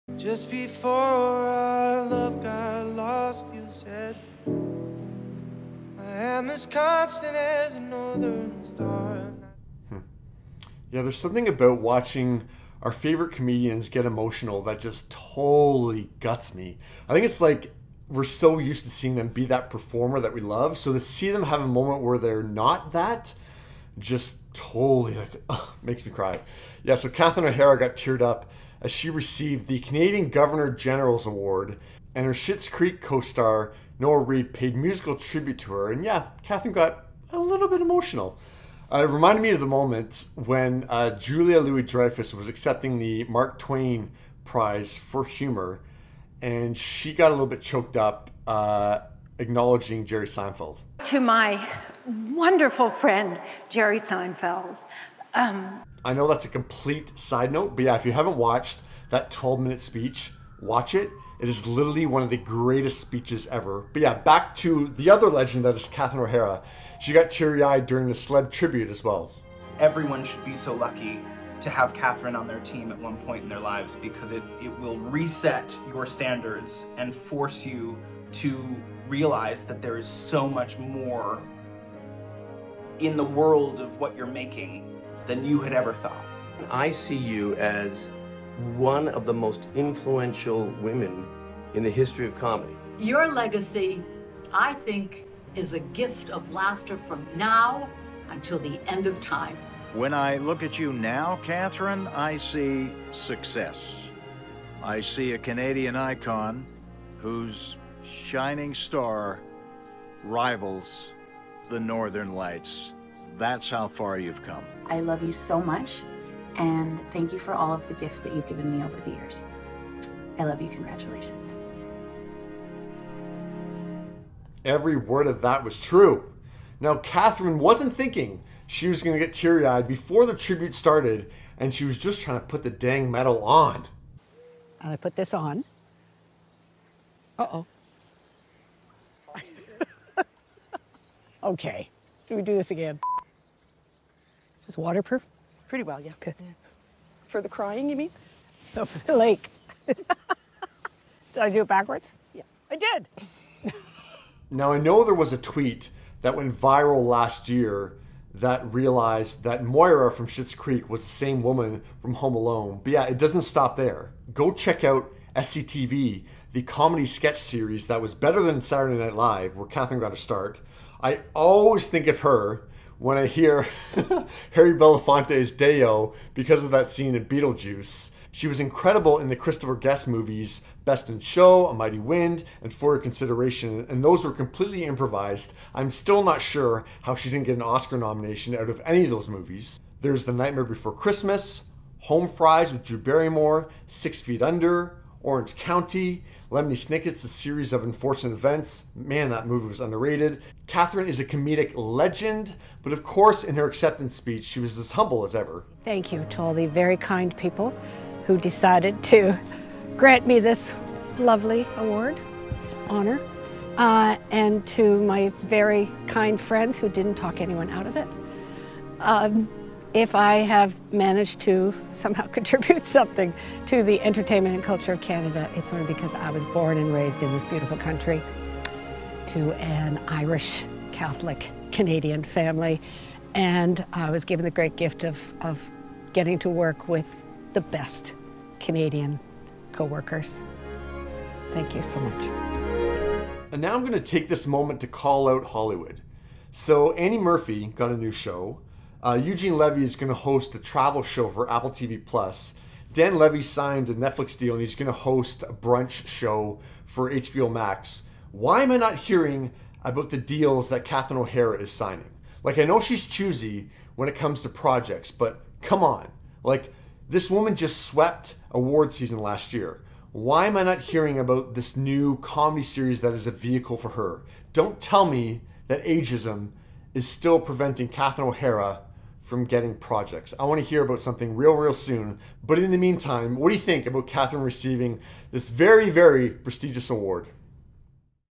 Catherine O'Hara Breaks Down During Award Tribute
Graeme O'Neil takes a look at Catherine O'Hara tearing up as she received the Canadian Governor General's Award, and was honoured by Eugene Levy, Martin Short, Andrea Martin, Annie Murphy, and Dan Levy.